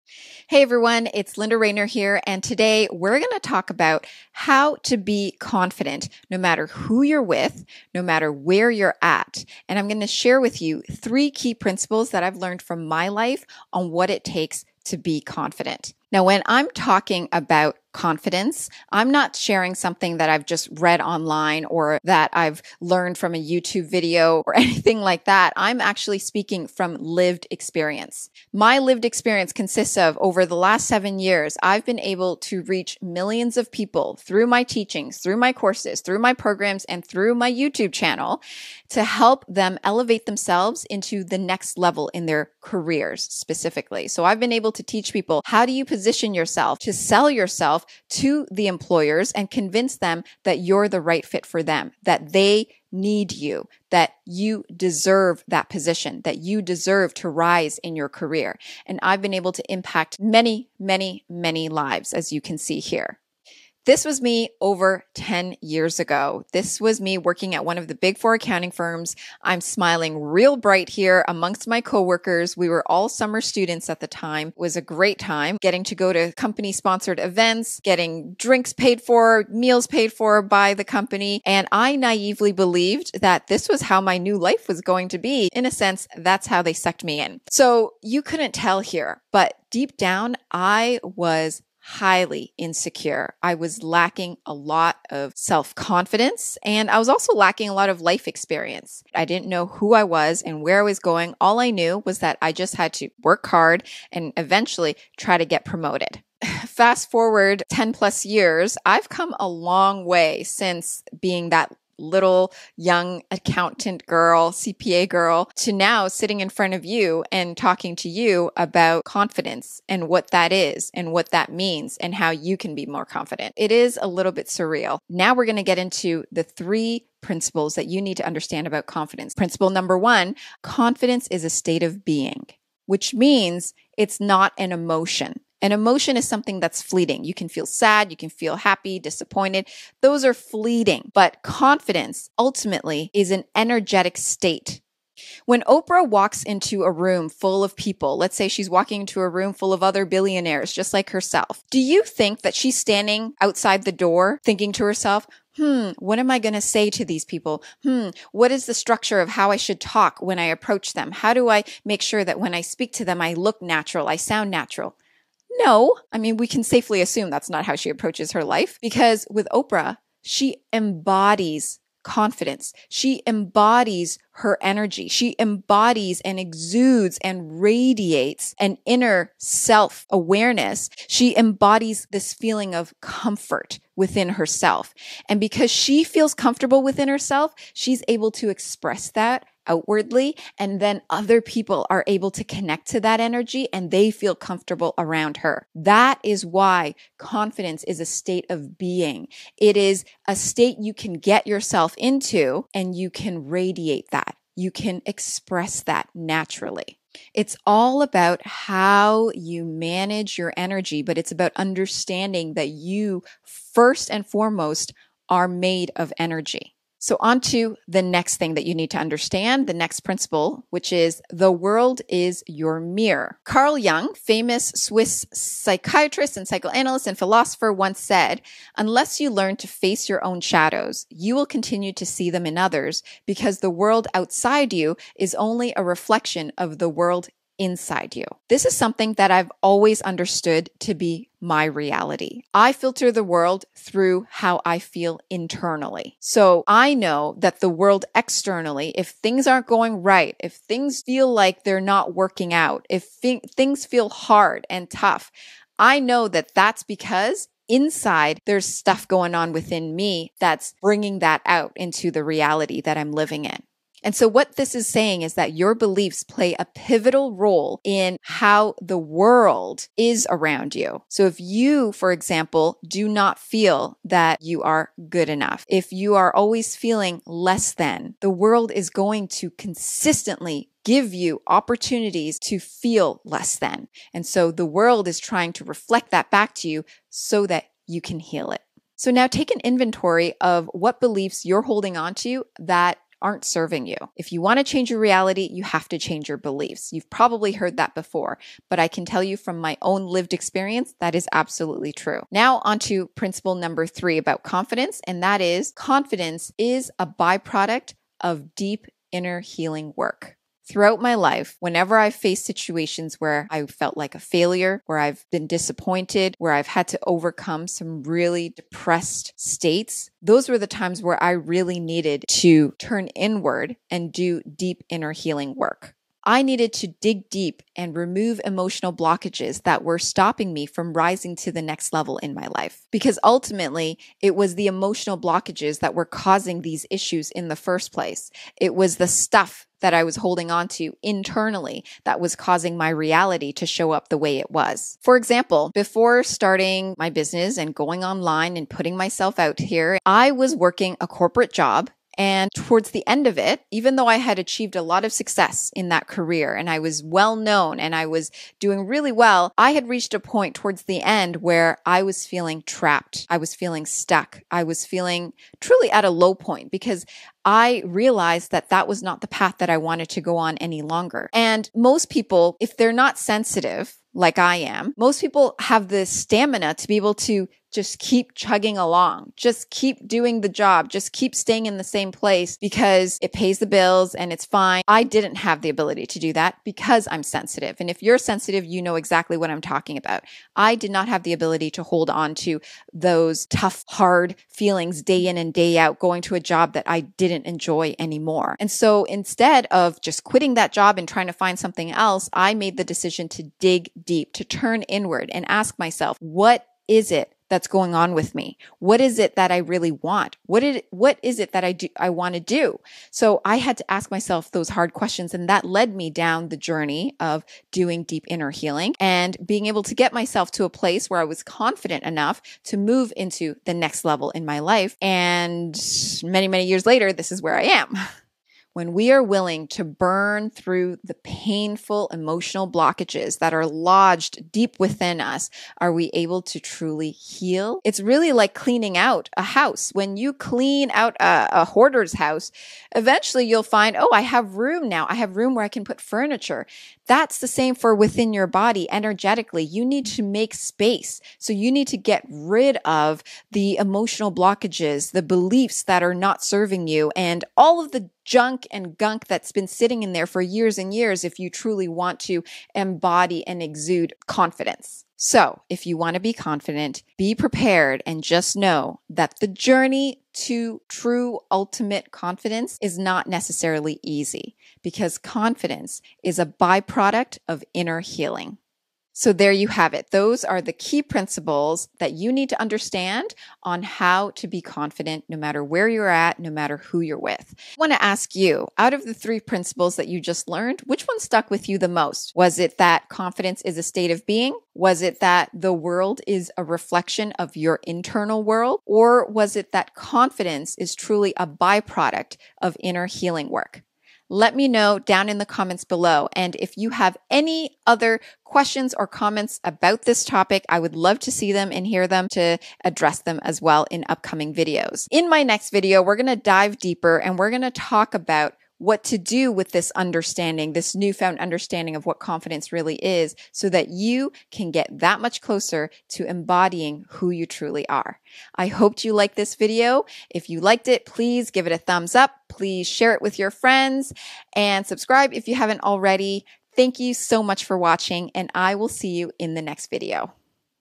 How to Feel Confident in Front of Anyone | 3 Key Principles
⭐ FREE MASTERCLASS: 4 Steps to Elevate Your Presence & Convince Hiring Managers to Pick You as the #1 Choice
Today you'll learn how to feel confident in front of anyone, how to be confident, how to express confidence and how truly confident people express themselves.